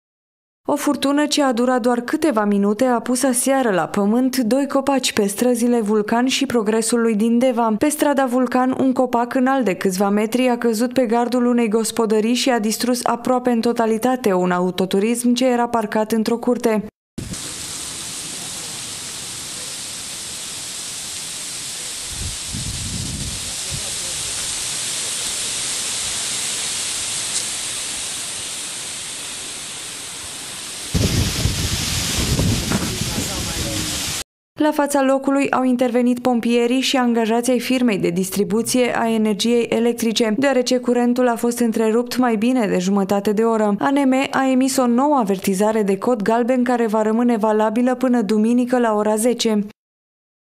DOI COPACI RUPȚI ȘI O MAȘINĂ AVARIATĂ
Rafalele puternice de vânt au rupt, aseară, doi copaci, în municipiul Deva